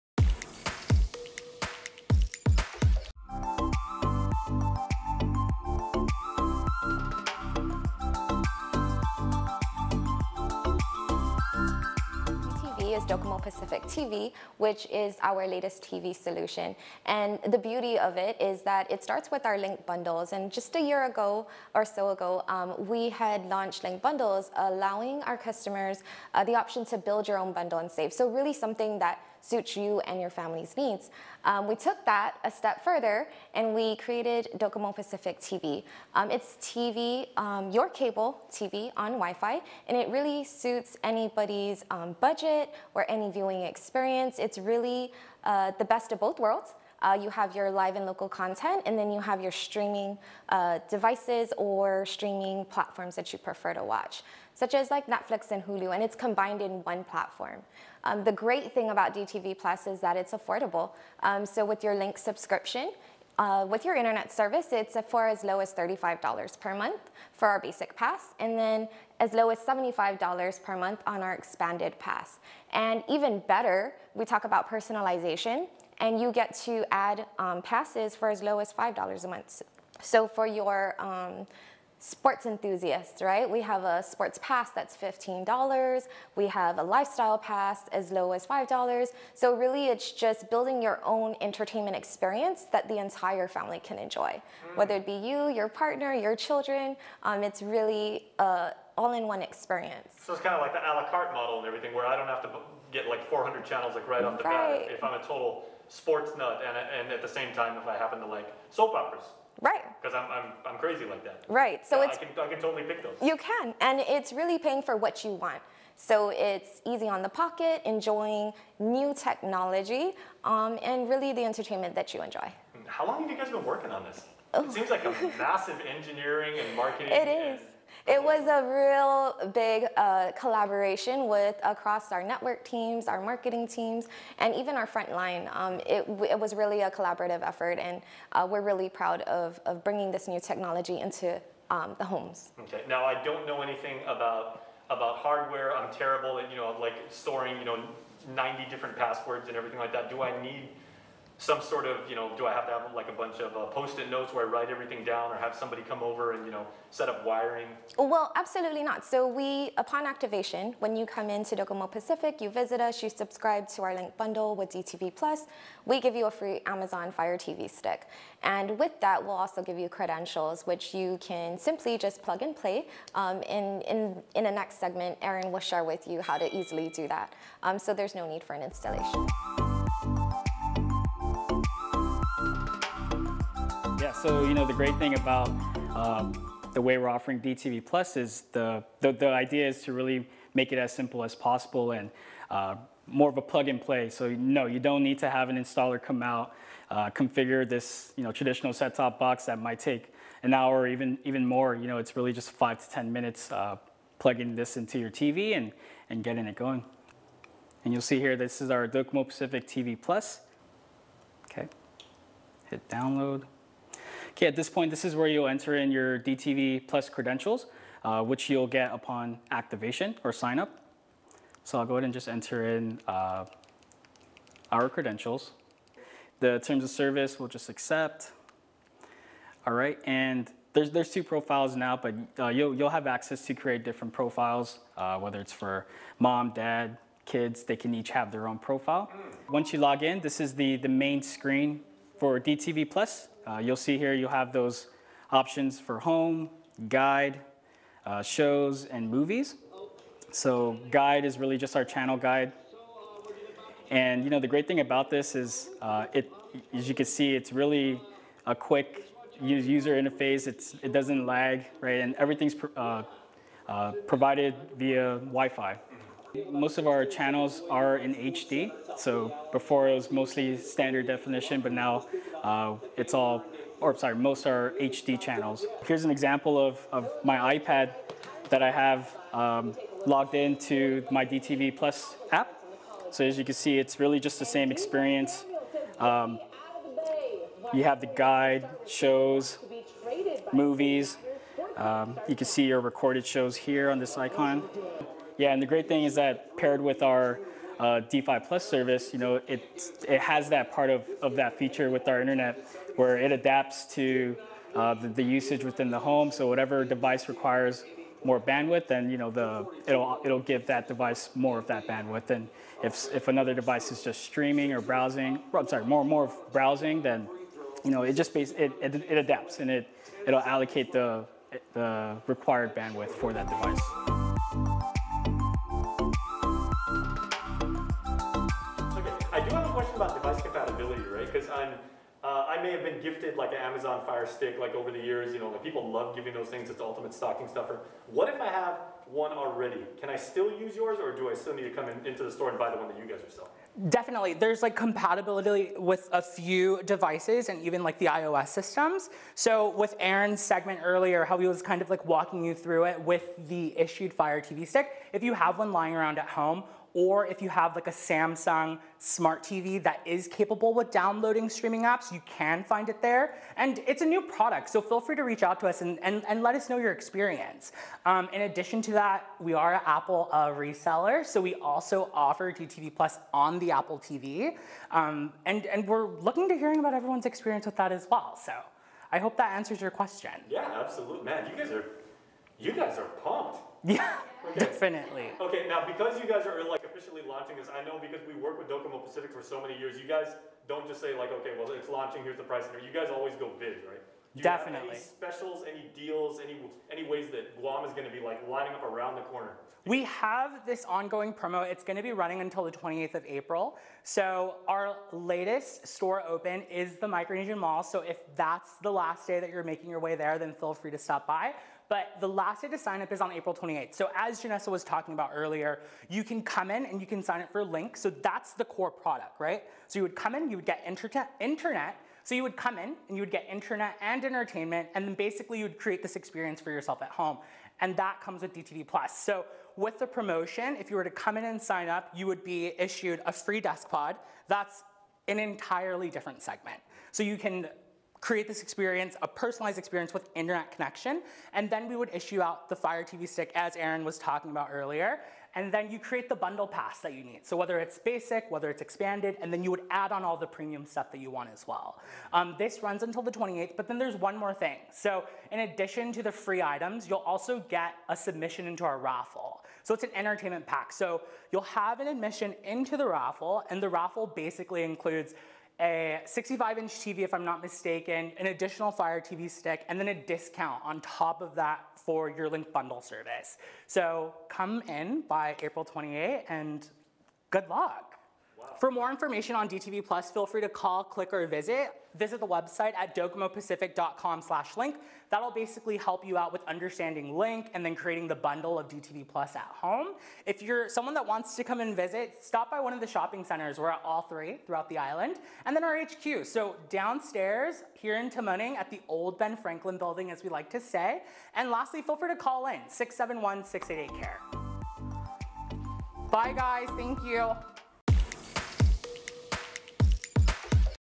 DOCOMO Pacific's product team gives you a primer on DTV+
The friendly folks on DOCOMO Pacific's product management team give you a tour of DTV+ - their platform for enjoying your favorite streaming services and cable TV program on your smart TVs, phones and tablets through a single stick!